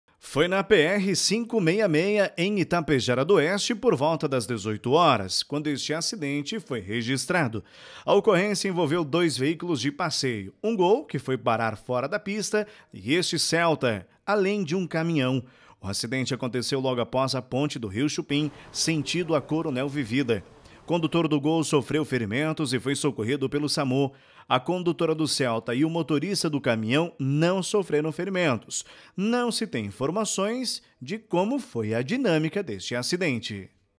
Um acidente envolveu dois carros e um caminhão em Itapejara D’Oeste
Acidente: Uma pessoa ficou ferida e foi socorrida pelo SAMU.